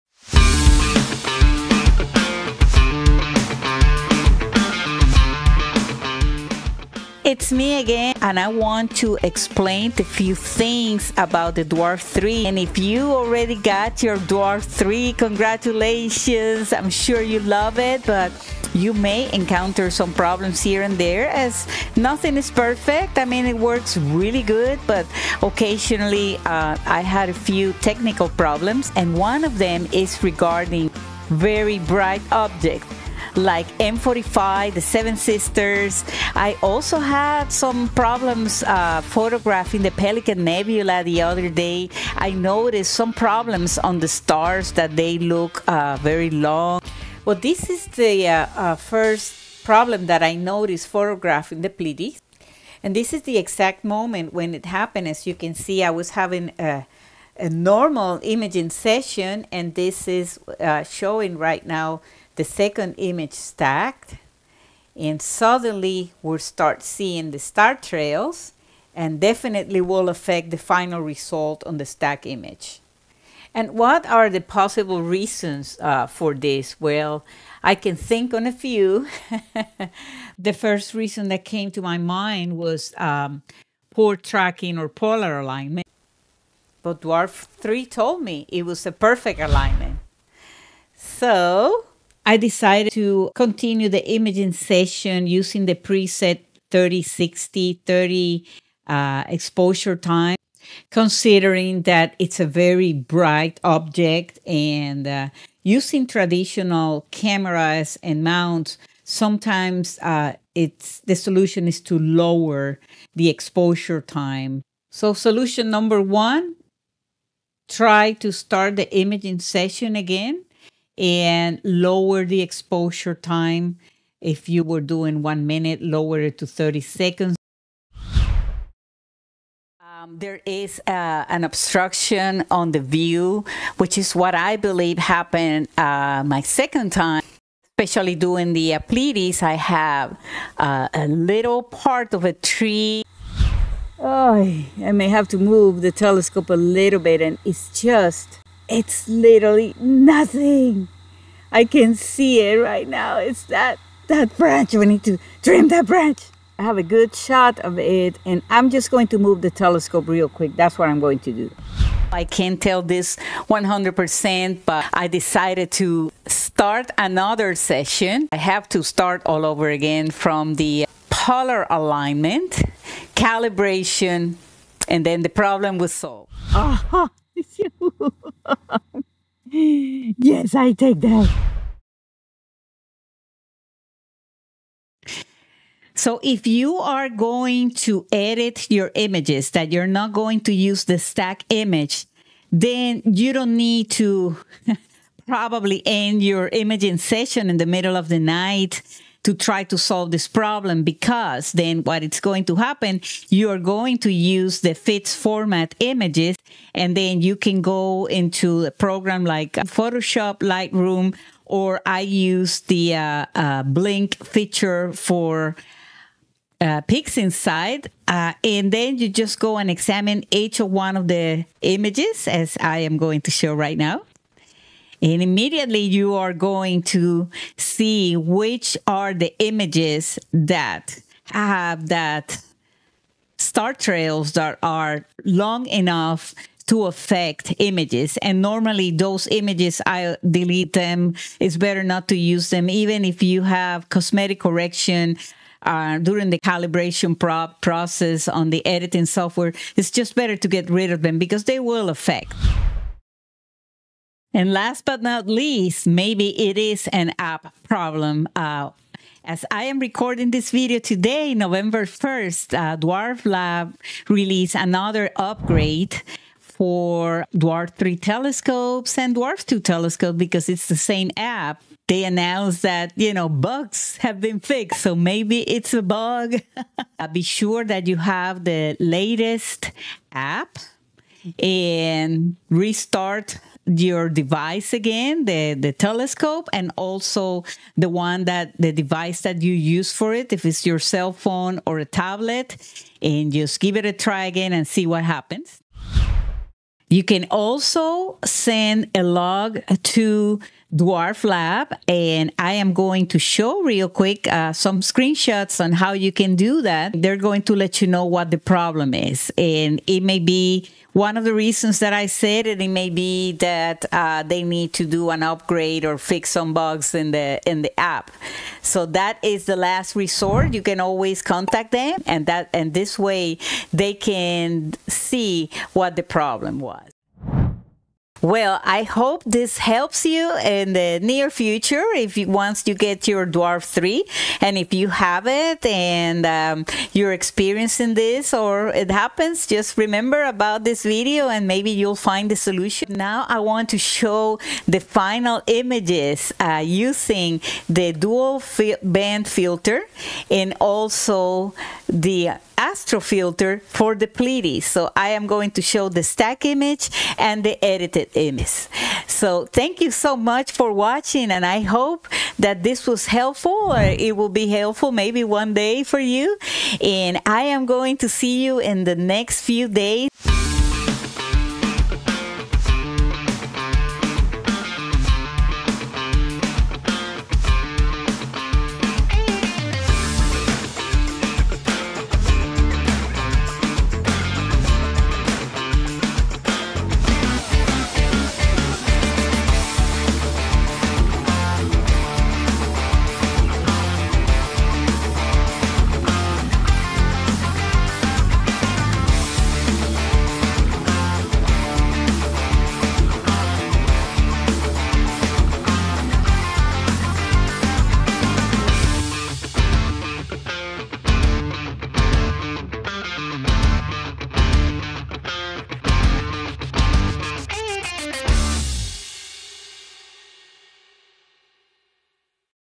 3 Easy Steps to Fix Your Dwarf 3 Telescope's Bad Stars Issue!
If you're having trouble with bad stars in your Dwarf 3 telescope, this video is for you! Learn how to troubleshoot and fix the issue for better images.

 dwarf 3 vs seestar s50 dwarflab seestar s50 vs dwarf 3